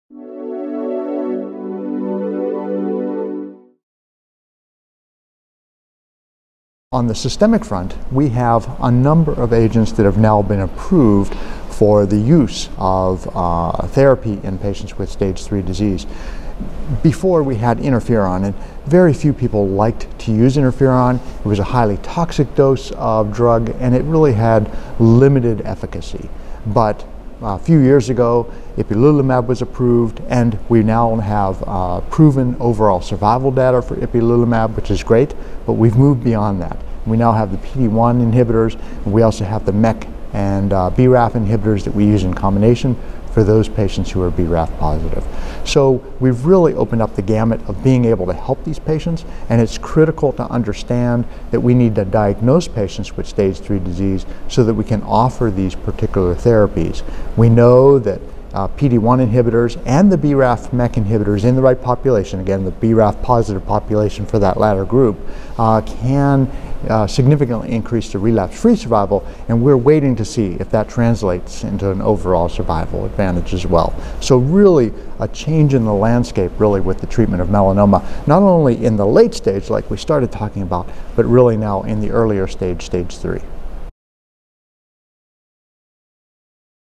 Dr. Olszanski Discusses Advances in Stage III Melanoma
Anthony J. Olszanski, MD, RPh, vice chair, Department of Hematology/Oncology, director, Early Drug Development Phase I Program, Fox Chase Cancer Center, discusses treatment advances in stage III melanoma.